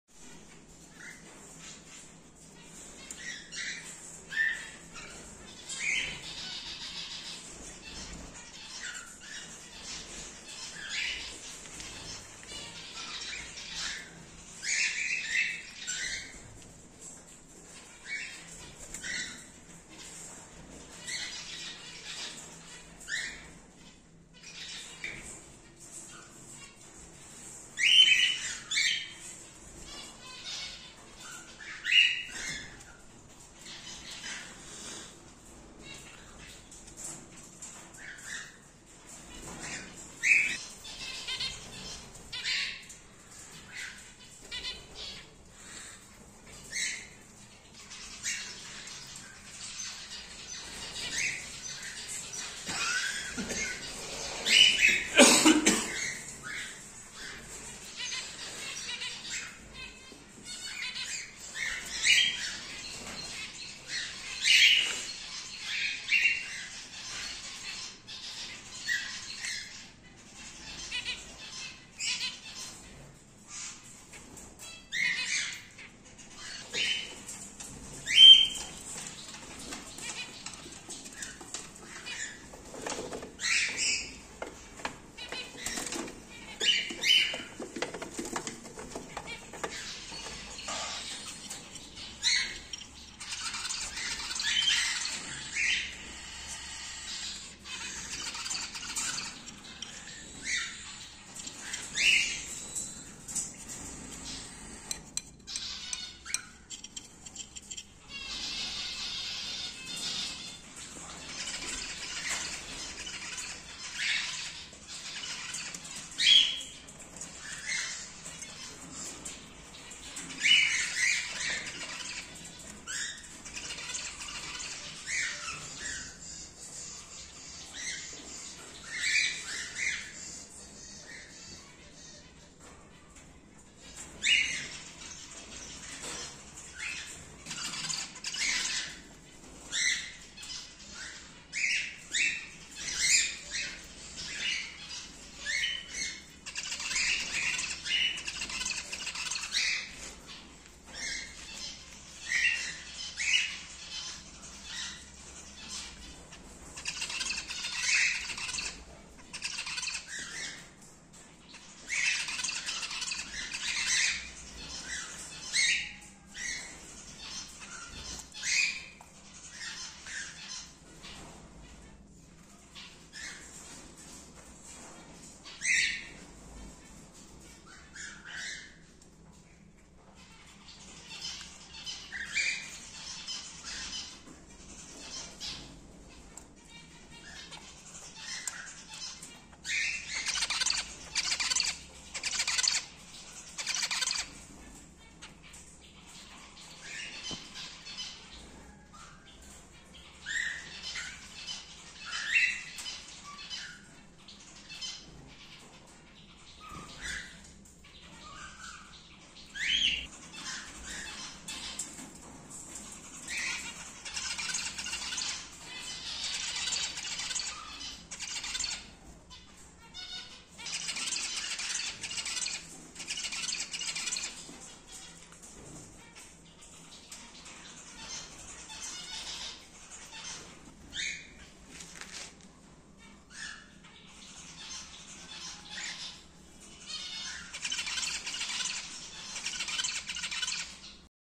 Cockatiel Colony Update | Exhibition Emerald Cockatiel Chicks | Alhamdulillah First day Out
Say Masha-Allah

Here you will see that cockatiel chicks progress (Alhamdulillah)
Alhamdulillah all chicks are doing well.
Alhamdulillah two more chicks come out of the box and enjoying first day out.
Now chicks are coming out of the box and having new experience of the new world